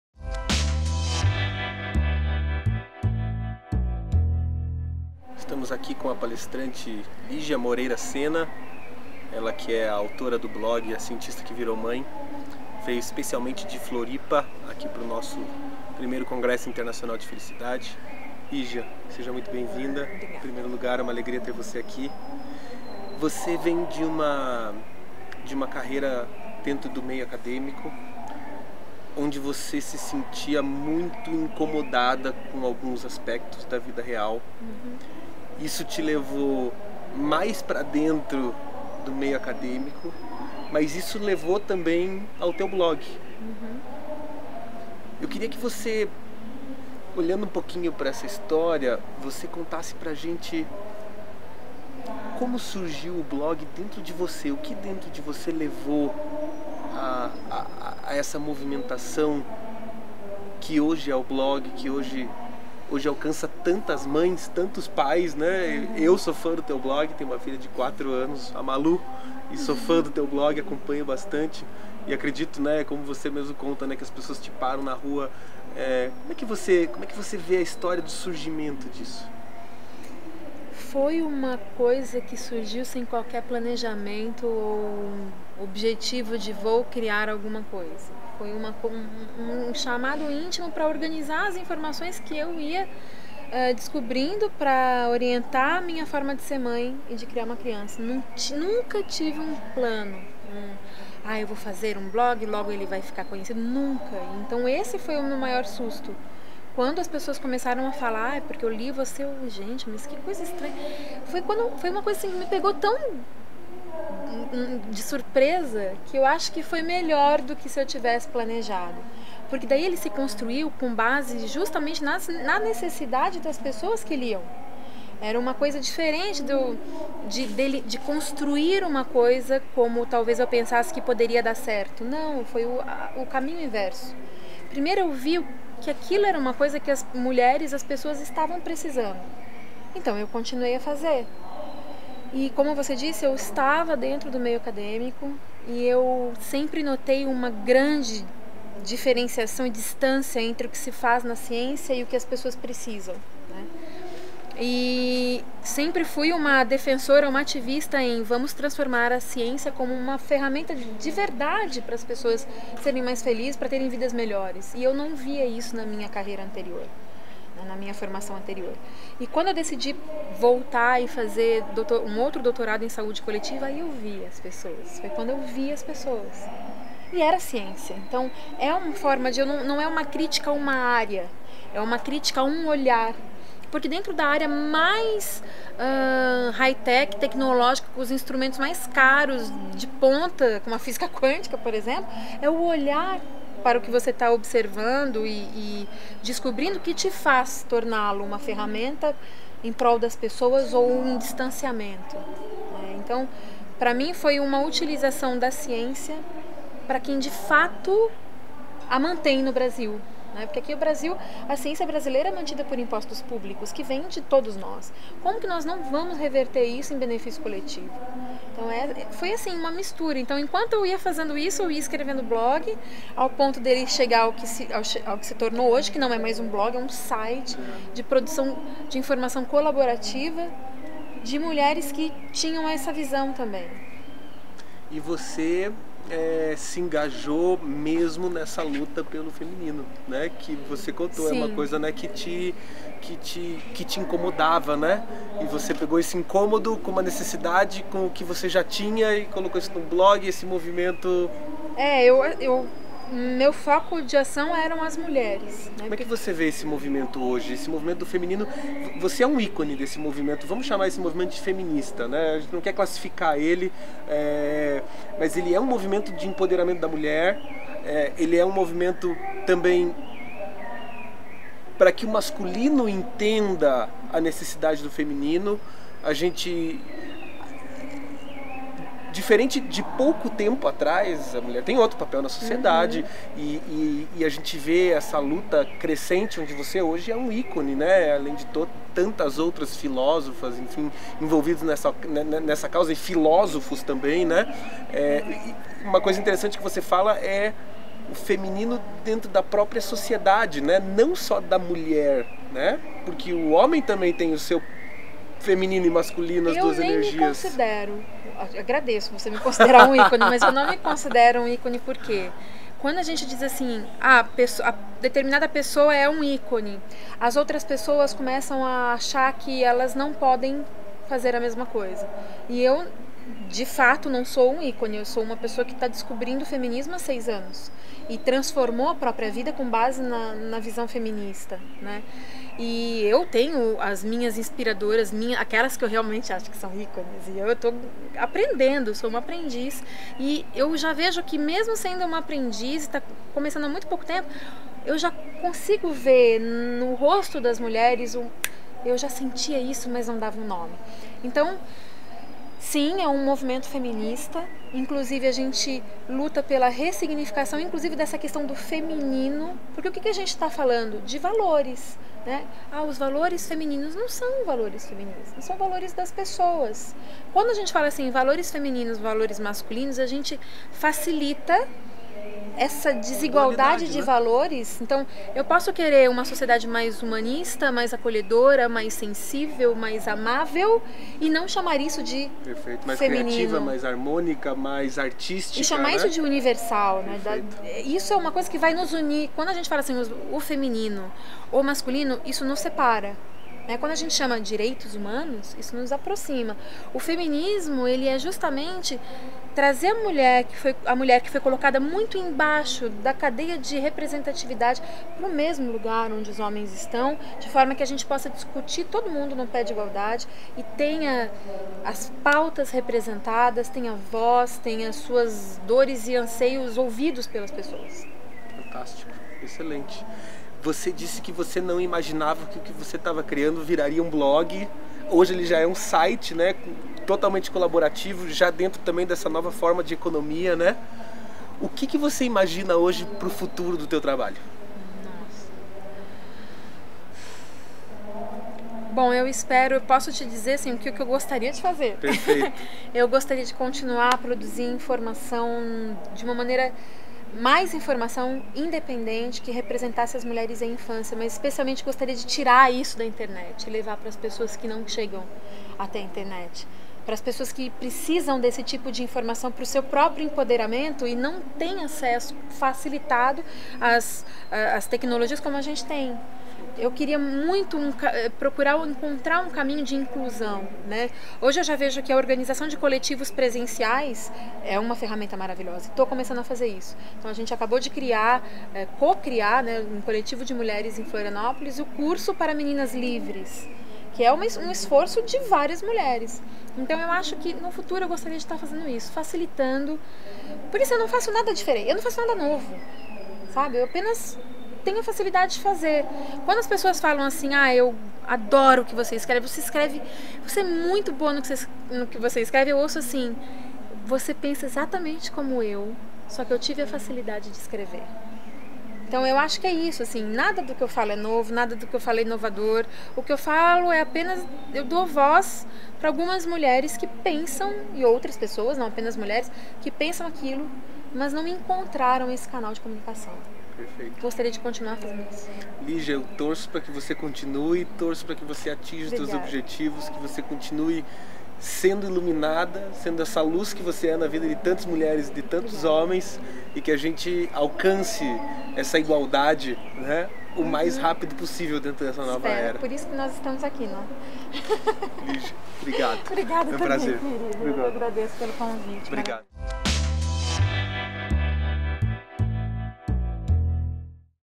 Entrevista com Ligia Moreira Sena - I Congresso Internacional de Felicidade
Em uma entrevista exclusiva com Gustavo Arns, idealizador do Congresso Internacional de Felicidade, Ligia Moreira Sena, bióloga, mestre e doutora em Ciências, fala sobre como surgiu a ideia de apoiar a luta pela ressignificação do papel da mulher na sociedade.

Criadora do blog “Cientista Que Virou Mãe”, ela comenta como surgiu a ideia do Blog, do envolvimento dela com ações que buscam o empoderamento da Mulher, sobre a identificação de muitas mulheres com os temas que ela expõe, as dificuldades que passam e como encontrar alternativas para mudar este cenário.
Mais que um movimento “feminista”, um movimento “pacífico”, que visa uma sociedade mais igual, mais humana e harmônica e que de fato, respeite a mulher com suas individualidades e fragilidades.